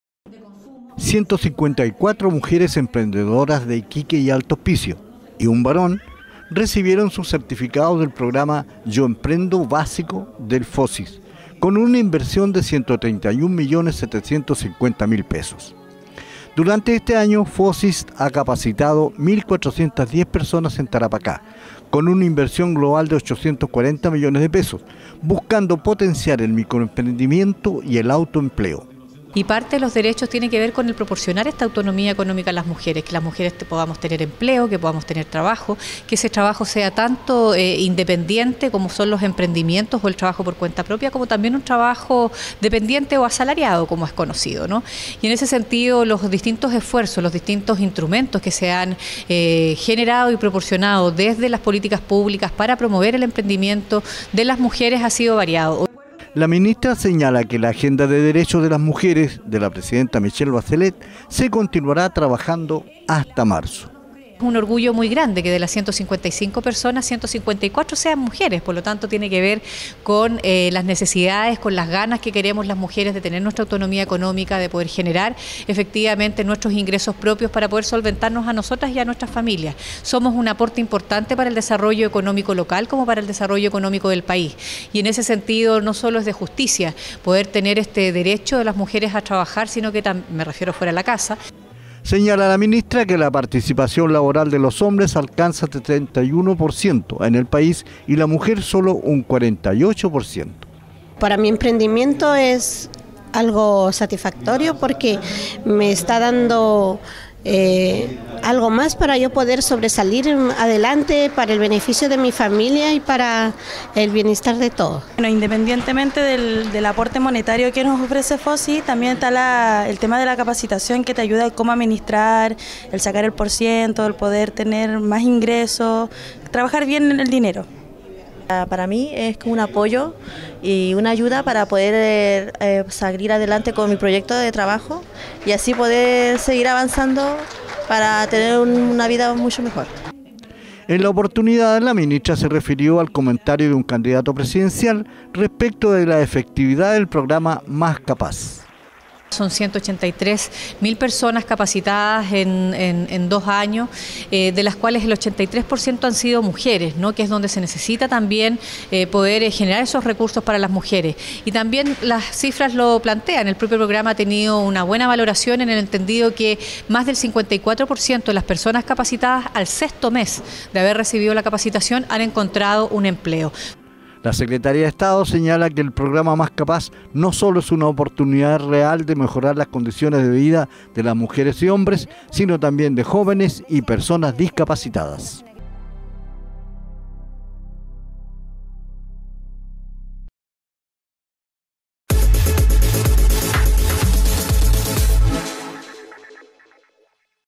FOSIS CERTIFICÓ 155 USUARIAS DE PROGRAMA YO EMPRENDO BÁSICO (2017.11.17) Lengua de señas
Mujeres emprendedoras de Iquique y Alto Hospicio, en número total de 154 damas y un varon, recibieron sus certificados del programa Yo emprendo Básico del Fosis, con una inversión de 131 millones 750 mil pesos. Durante este año Fosis ha capacitado a 1.410 personas en Tarapacá, con una inversión global de 840 millones de pesos, buscando potenciar el microemprendimiento y el autoempleo.